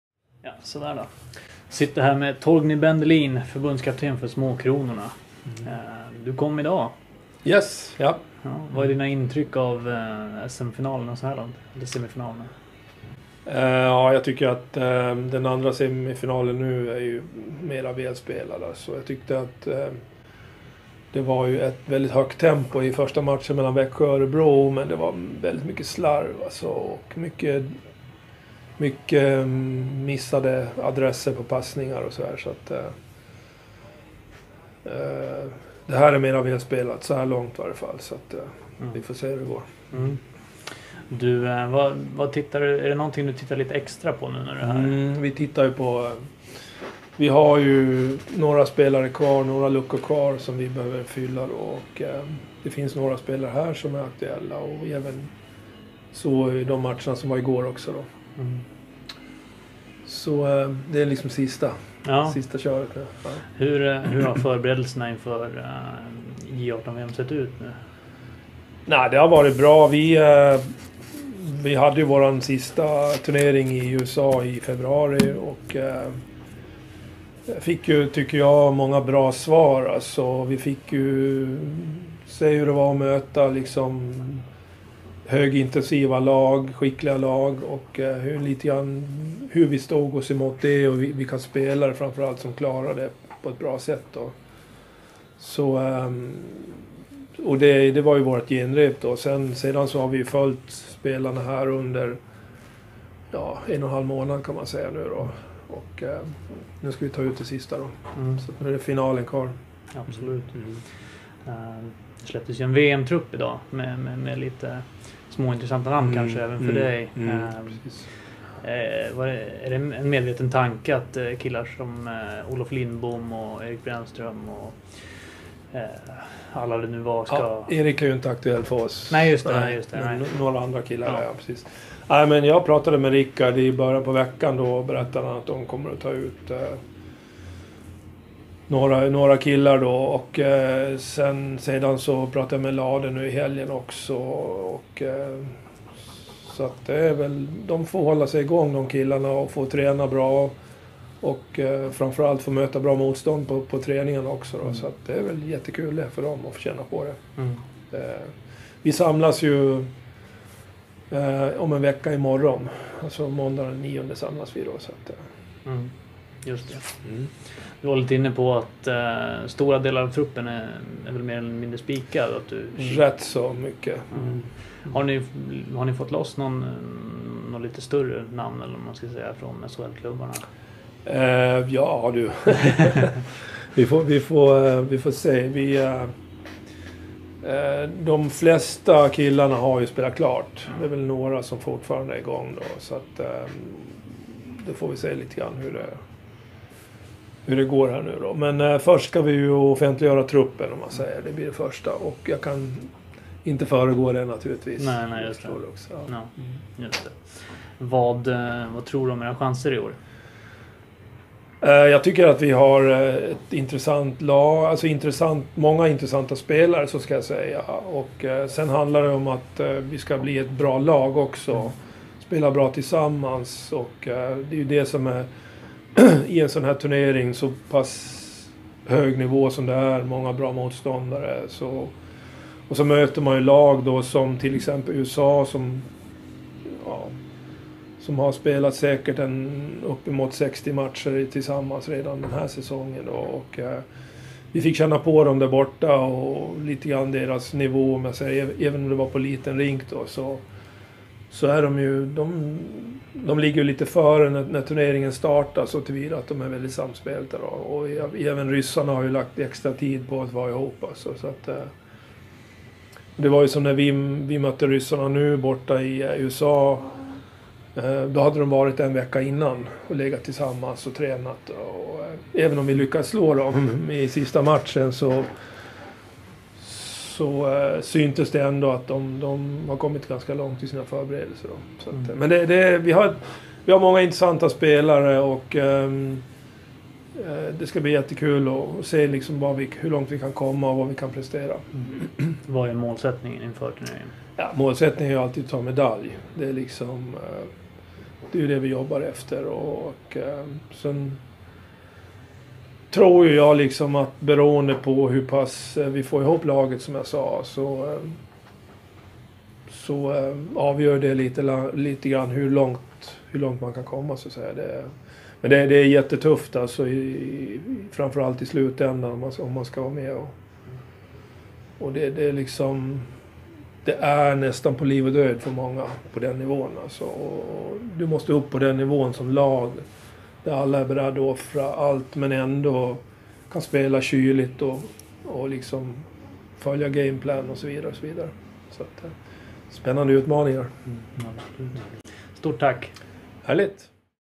Bendelin inför U18-VM: "På liv och död"
Hockeymagasinet tog pulsen på förbundskaptenen Torgny Bendelin i Nyköping inför kommande U18-VM.